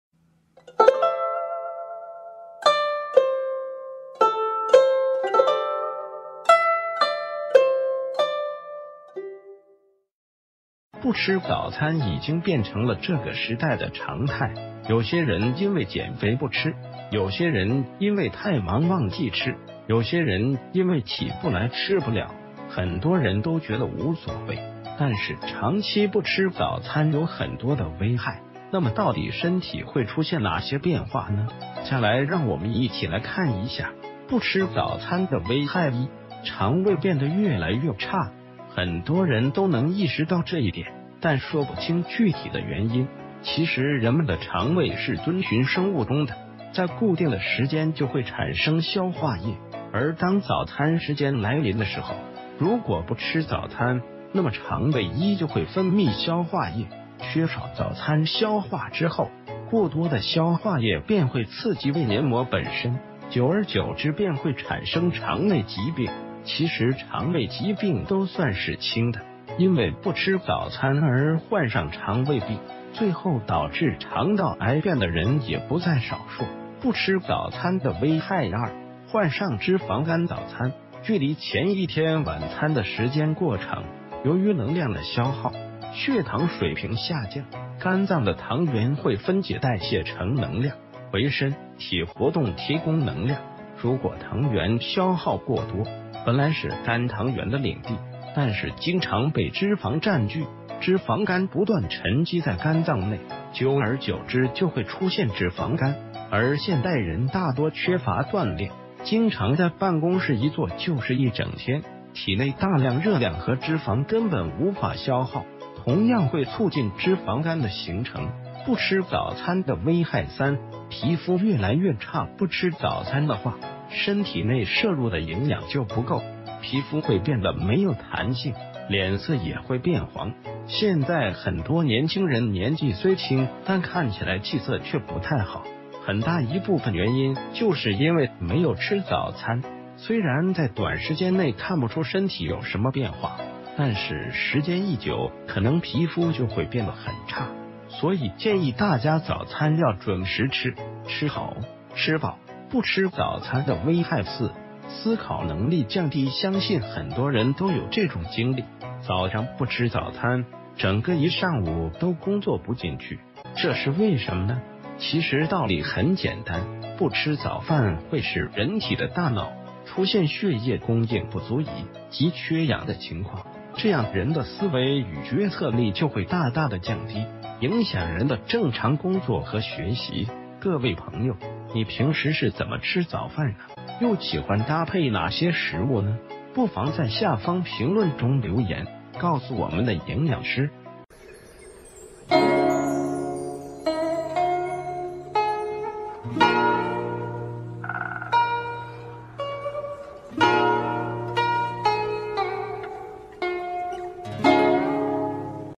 營養師實話告訴你：長期不吃早飯，這四種疾病都會找到你
不吃早餐已經變成了這個時代的常態，有些人因為減肥不吃，有些人因為太忙忘記吃，有些人因為起不來吃不了，很多人都覺得無所謂，但是長期不吃早餐有很多的危害，那么到底身體會出現哪些變化呢？下來讓我們一起來看一下。

不吃早餐的危害1：腸胃變的越來越差

很多人都能意識到這一點，但說不清具體的原因，其實人們的腸胃是遵循生物鐘的，在固定的時間就會產生消化液。而當早餐時間來臨的時候，如果不吃早餐，那么腸胃依舊會分泌消化液，缺少早餐消化之后，過多的消化液便會刺激胃黏膜本身，久而久之便會產生腸胃疾病。

其實腸胃疾病都算是輕的，因為不吃早餐而患上腸胃病，最后導致腸道癌變的人也不在少數。

不吃早餐的危害2：患上脂肪肝

早餐距離前一天晚餐的時間過長，由于能量的消耗，血糖水平下降，肝臟的糖原會分解代謝成能量，為身體活動提供能量。如果糖原消耗過多，本來是肝糖原的領地，但是經常被脂肪“占據”，脂肪肝不斷沉積在肝臟內，久而久之就會出現脂肪肝。

而現代人大多缺乏鍛煉，經常在辦公室一坐就是一整天，體內大量熱量和脂肪根本無法消耗，同樣會促進脂肪肝的形成。

不吃早餐的危害3：皮膚越來越差

不吃早餐的話，身體內攝入的營養就不夠，皮膚會變得沒有彈性，臉色也會變黃。現在很多年輕人年紀雖輕但看起來氣色卻不太好，很大一部分原因就是因為沒有吃早餐，雖然在短時間內看不出身體有什么變化，但是時間一久可能皮膚就會變的很差。所以建議大家早餐要準時吃，吃好，吃飽。

不吃早餐的危害4：思考能力降低

相信很多人都有這種經歷，早上不吃早餐，整個一上午都工作不進去，這是為什么呢？其實道理很簡單，不吃早飯會使人體的大腦出現血液供應不足以及缺氧的情況，這樣人的思維與決策力就會大大的降低，影響人的正常工作和學習。

各位朋友，你平時是怎么吃早飯呢？又喜歡搭配哪些食物呢？不妨在下方評論中留言，告訴我們的營養師。